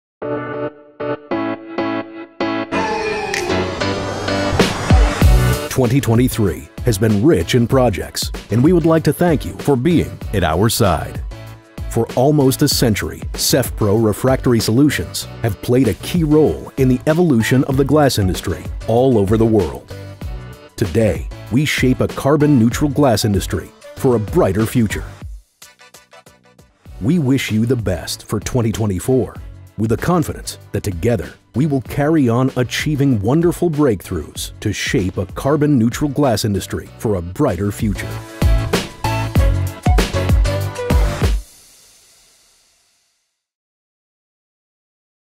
SEFPRO Greetings for 2024
In this new year, SEFPRO extends its warmest wishes. May 2024 be a year filled with success, innovation, and prosperity. We thank our customers, partners and collaborators for their continued trust. SEFPRO remains dedicated to excellence in the refractory materials for glass industry, and we look forward to sharing new successes and achievements throughout this year.
A beautiful year 2024 to all, from SEFPRO.